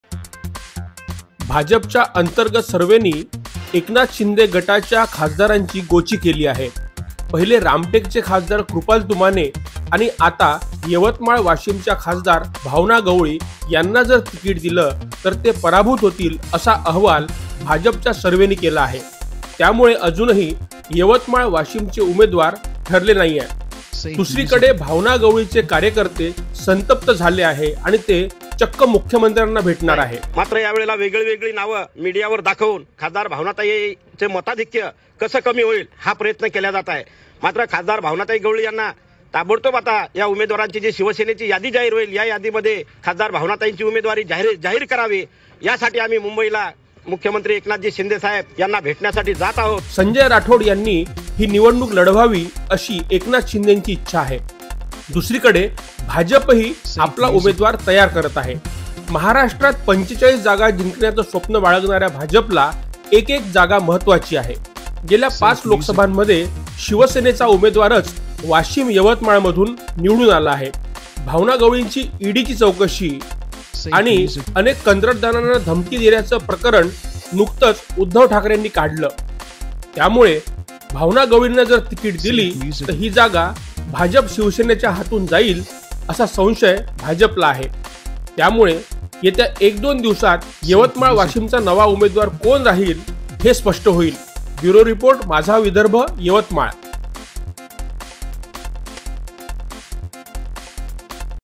Yavatmal washim Loksabha भाजपच्या सर्वेमुळे भावना गवळींचा पत्ता कट होणार ? नवा उमेदवार कोण
लोकसभा निवडणूक 2024 भाजपच्या अंतर्गत सर्वेने शिंदे गटाच्या उमेदवारांची गोची केली असून रामटेकचे खासदार कृपाल तुमाने आता यवतमाळ वाशिम च्या खासदार भावना गवळी यांची उमेदवारी संभ्रमात आणली आहे. भाजप तर्फे असा सर्वे काढण्यात आला की, भावना गवळी यांना तिकीट दिल्यास ते पराभूत होतील त्यामुळे यवतमाळ वाशिम येथून संजय राठोड यांनी निवडणूक लढवावी अशी एकनाथ शिंदे यांची इच्छा आहे .येथील उमेदवारी साठी भाजप आपला प्रतिनिधी तयार करत आहे. गवळी यांच्यावर असलेले आरोप बघता उमेदवारीसाठी आता भाजपने कुठलाही निर्णय दिलेला नाही यामुळे भावना गवळीच्या कार्यकर्त्यांमध्ये नाराजगी आहे... आणि ते सुद्धा भावना गवळींच्या उमेदवारीसाठी मागणी करत आहेत#mahrashtrapolitics